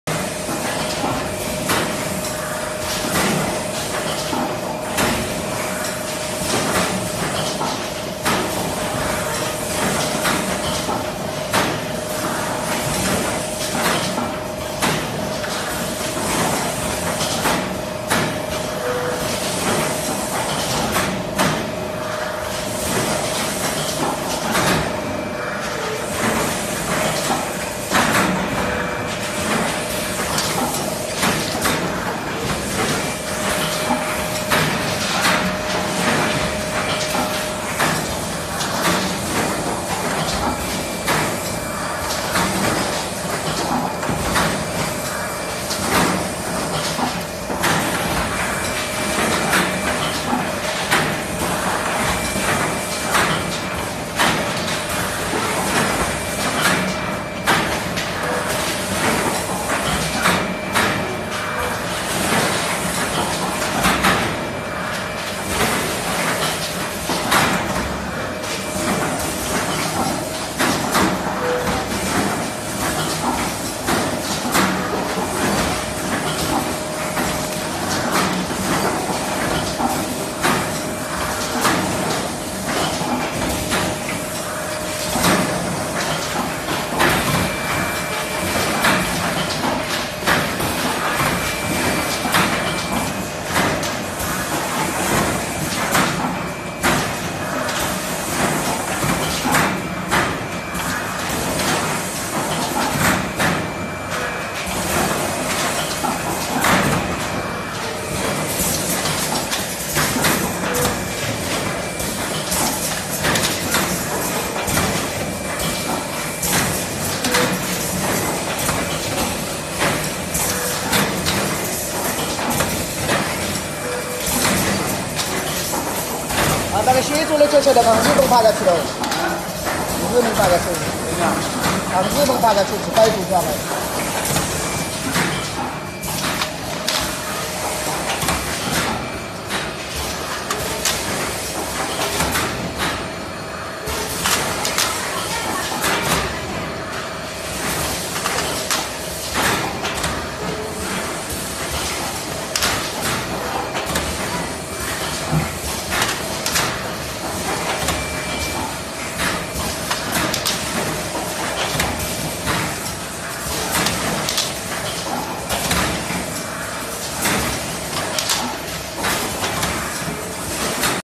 Auto Cup Fill & Seal Machine
Automatic Cup/Pot Fill & Seal Machine. Magazine style lids, speed up to 30cycles/min multiply by number of lanes. Each lane has independent sensor. No cup no filling or lidding.